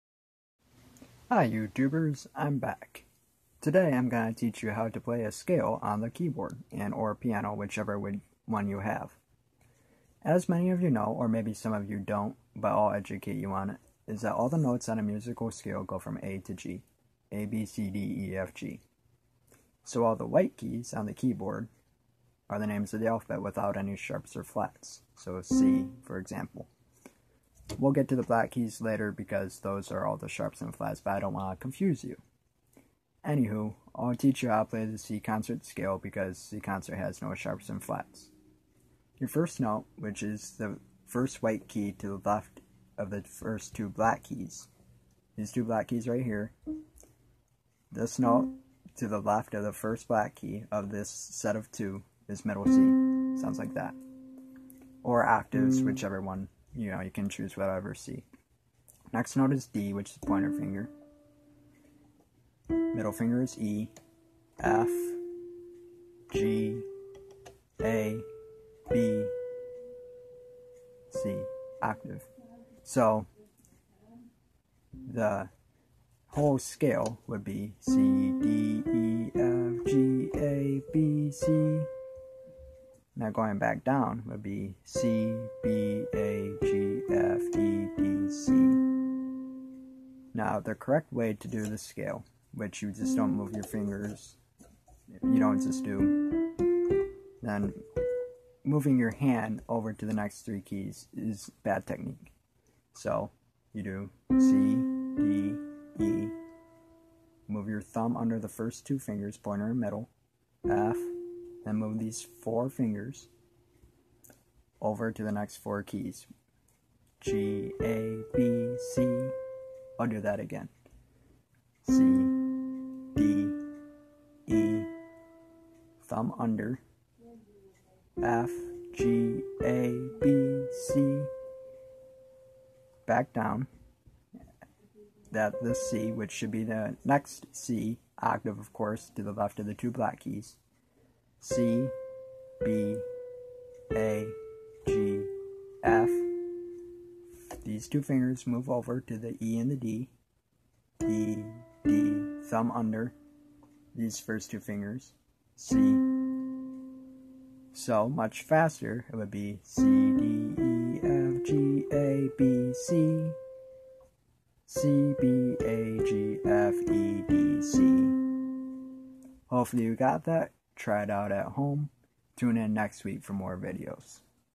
Learning White Keys and C Concert Scale
This video teaches you what a white key is and shows you how to play a C major scale.